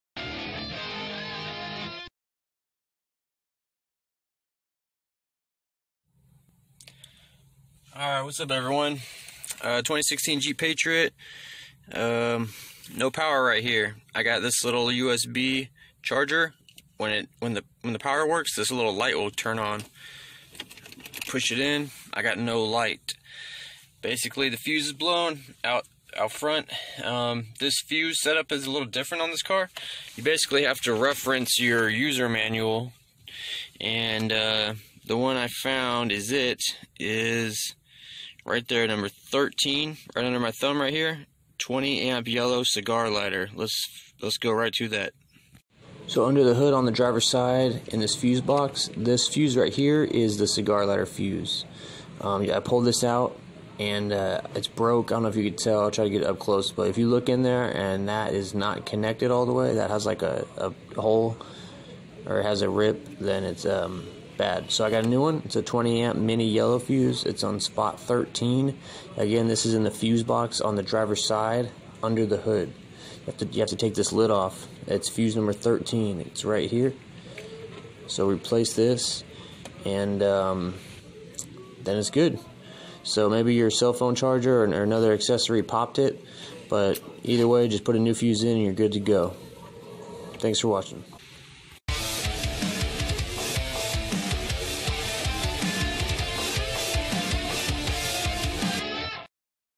2016 JEEP PATRIOT POWER PORT / CIGAR LIGHTER INOP
Video tutorial on how to fix the cigar lighter or power port on a 2016 Jeep Patriot. The cigar lighter was not working and basically it just had a popped fuse. The fuse for the cigar lighter is under the hood in the fuse box it is number 13

(Retro / Anime / Video game + more) - murals, paintings, Prints & more